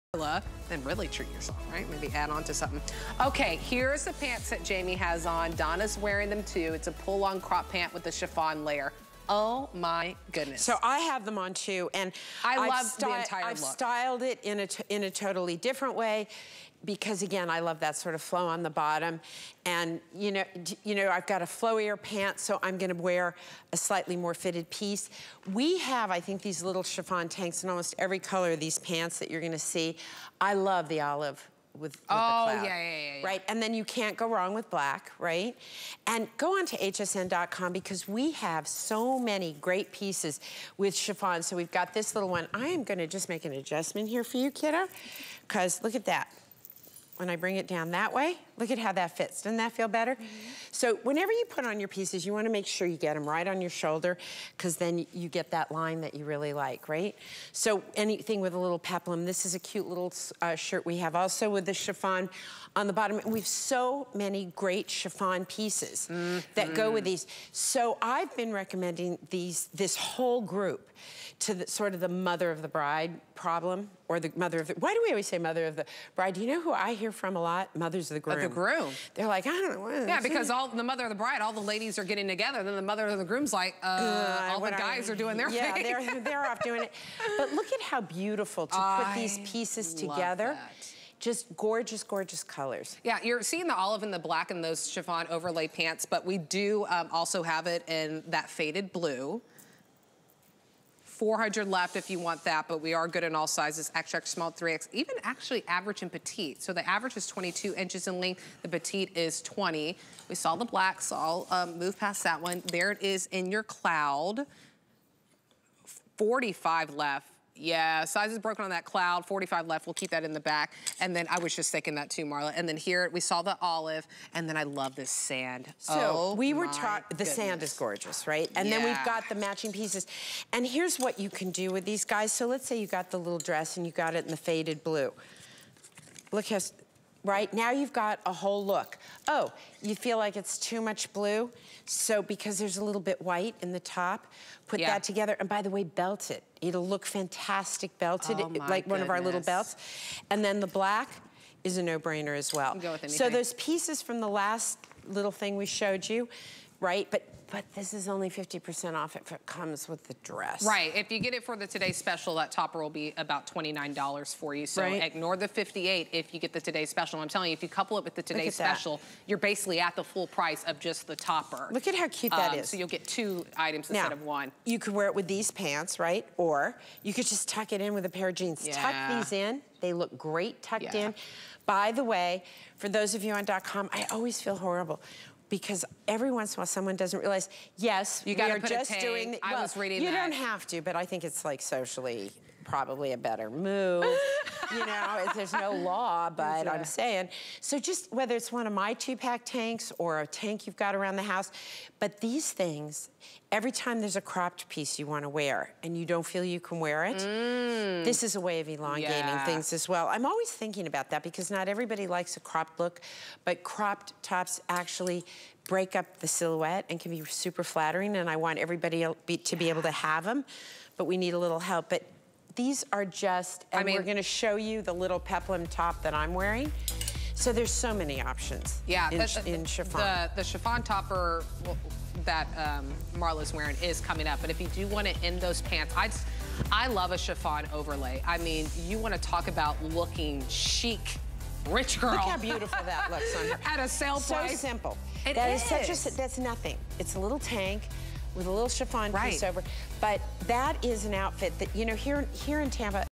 WynneLayers PullOn Crop Pant with Chiffon Overlay
Break free of the everpopular skinny jean silhouette and opt for a more dramatic look with these doublelayered pants from WynneLayers. They have just as much movement as any swingy skirt, and the chiffon overlay makes them extra fabulous.